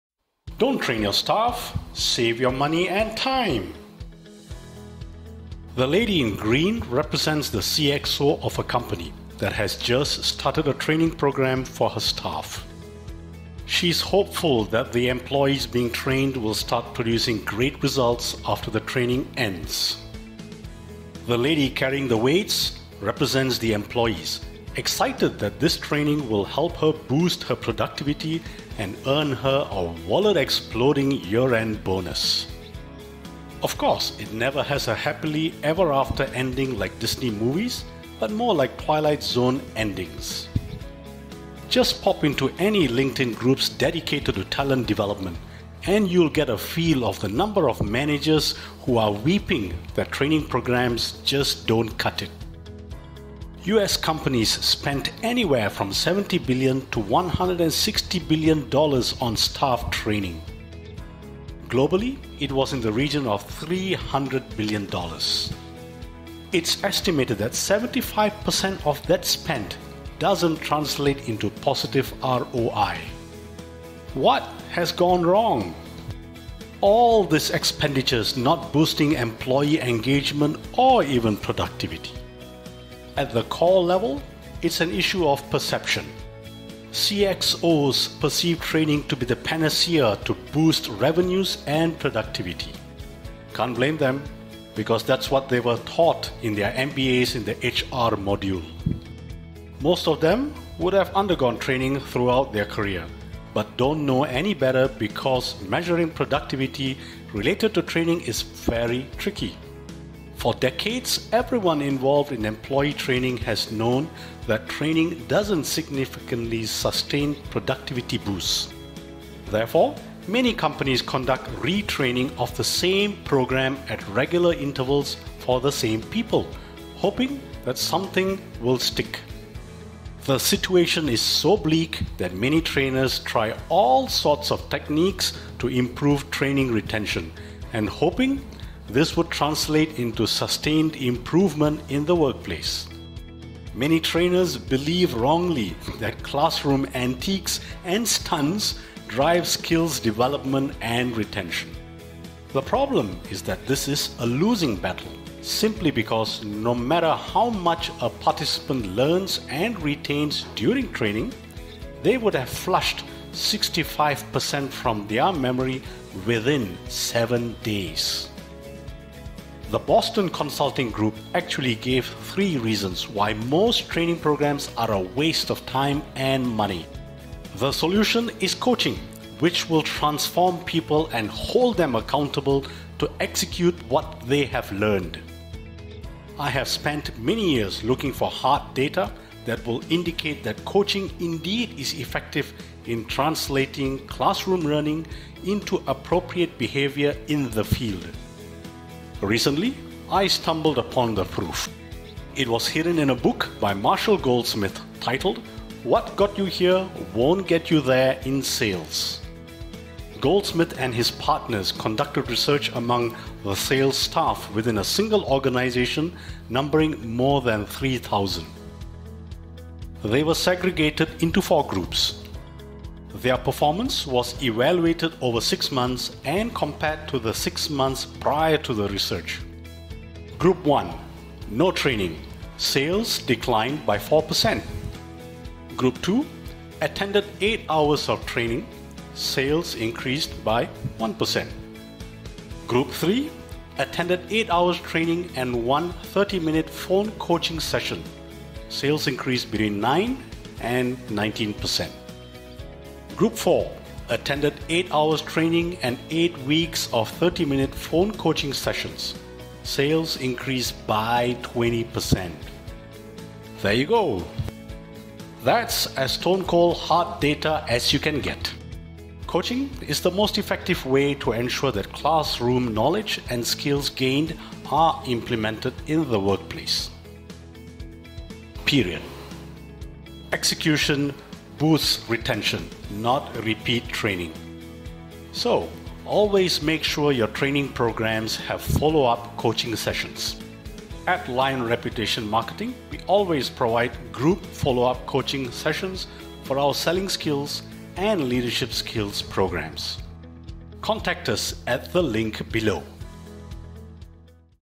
Dont Train Your Staff - Save Your Money & Time
Don't train your staff. It's a waste of your money and time, unless you do one thing after the training.

3:30 Research into sales' team's performance showed the one thing that needs to be done to give you a positive ROI.